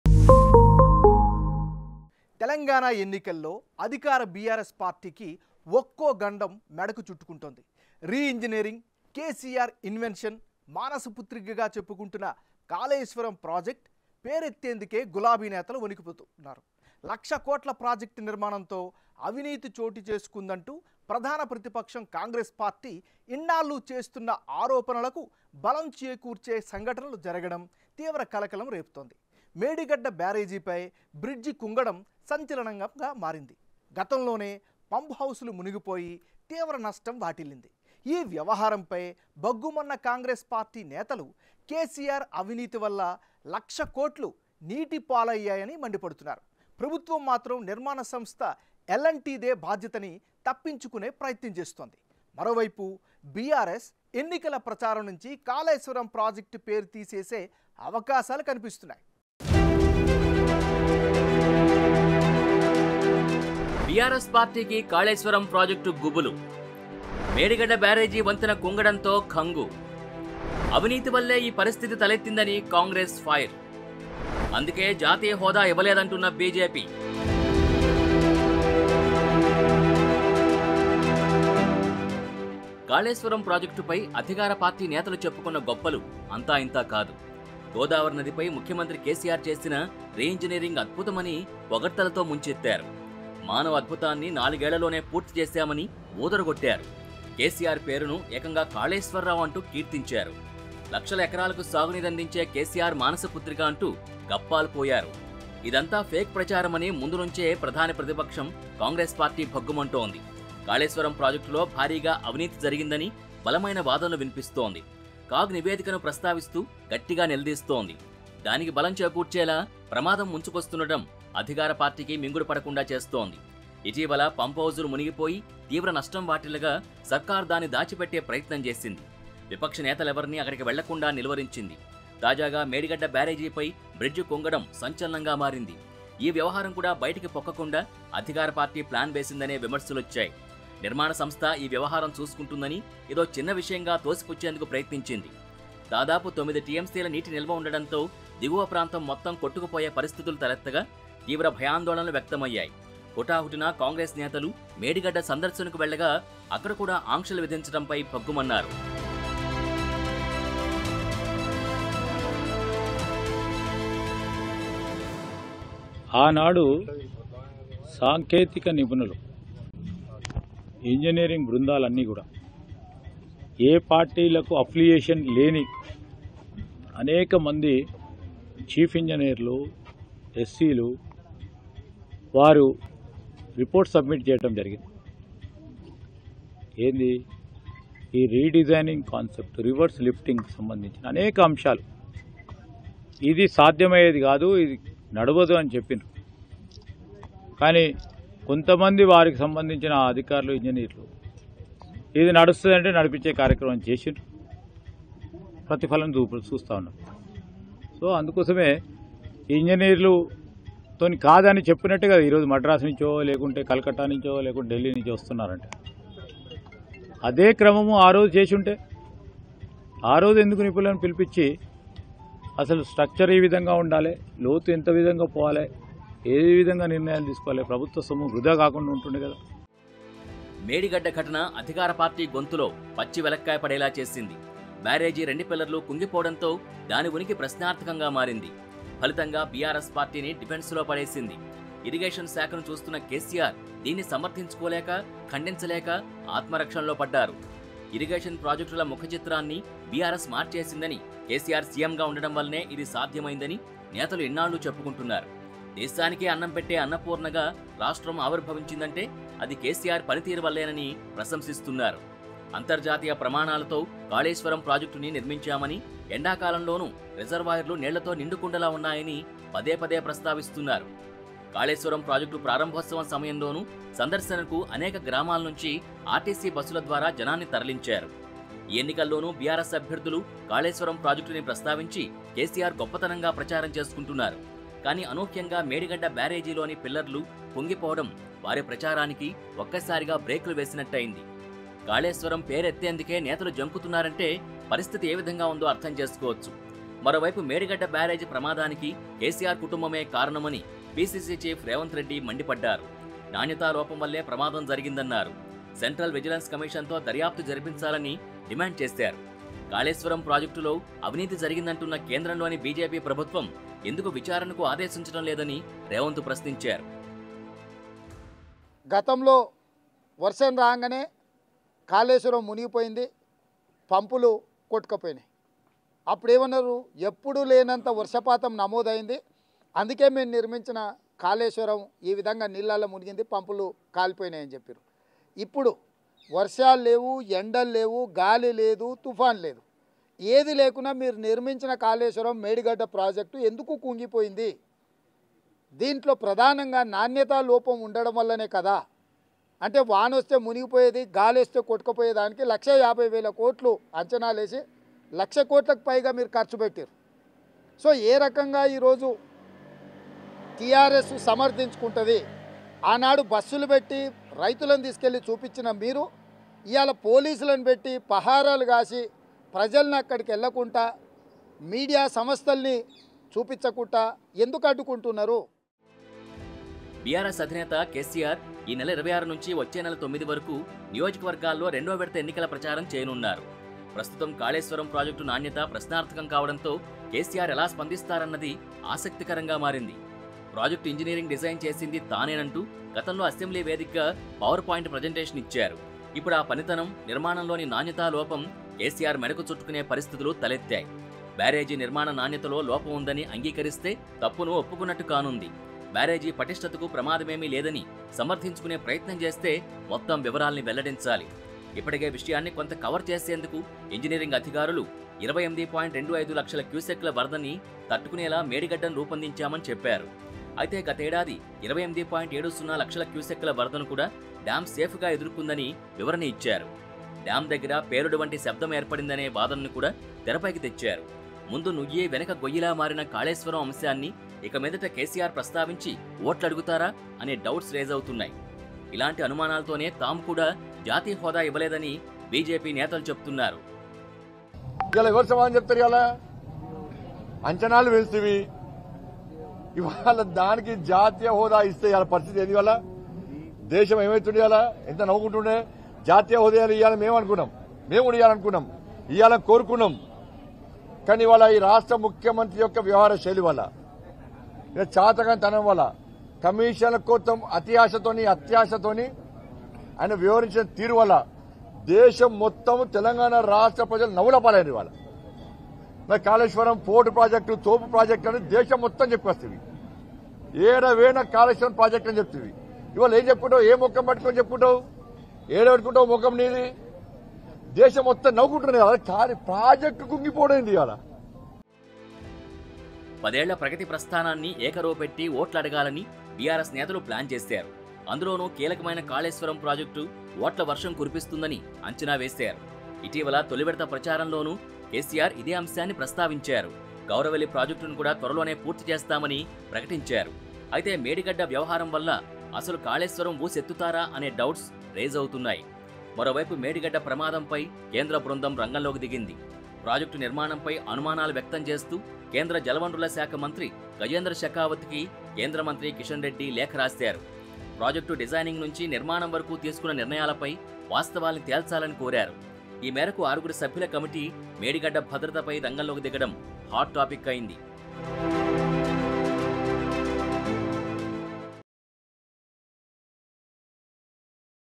Medigadda Barrage in Danger : బీఆర్ఎస్ పార్టీ గుండెల్లో కాళేశ్వరం ప్రాజెక్టు గుబులు | Kaleshwaram
Medigadda Barrage in Danger : బీఆర్ఎస్ పార్టీ గుండెల్లో  కాళేశ్వరం ప్రాజెక్టు గుబులు | Kaleshwaram | BIG TV Telugu